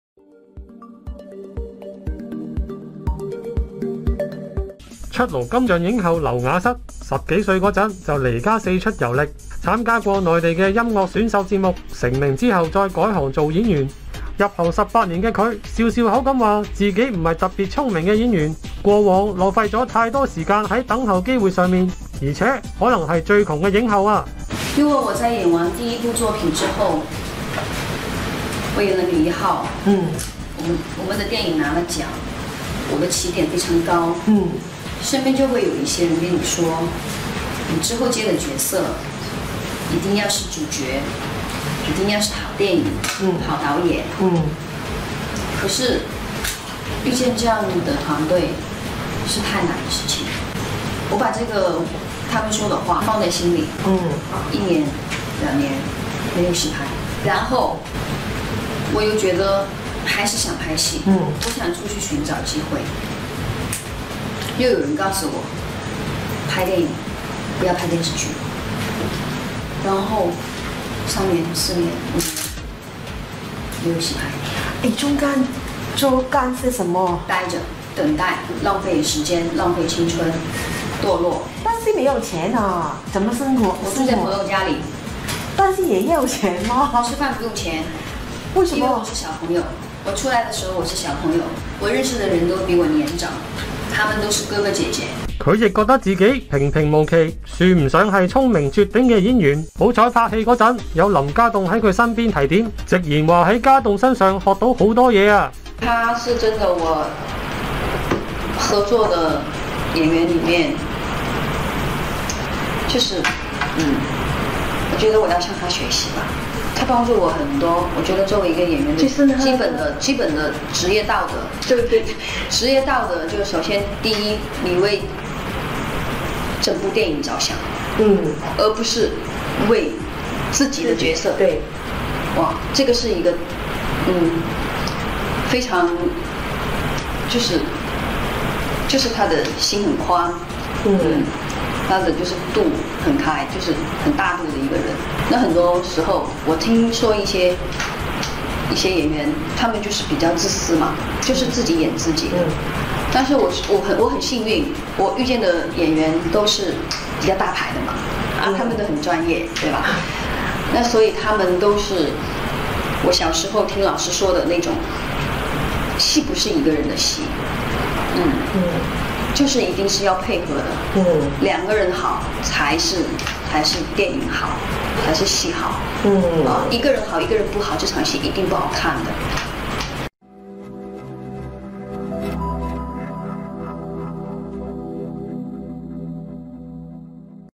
劉雅瑟入行18年經歷墮落迷失 自封最窮影后
出爐金像影后劉雅瑟，十多歲便離家四出「遊歷」，參加內地音樂選秀節目成名再「改行」當演員，背景與前輩周迅相似。她笑指自己不是特別聰明的演員，過往浪費太多時間在等候機會上，且可能是「最窮影后」，但堅持不改初心！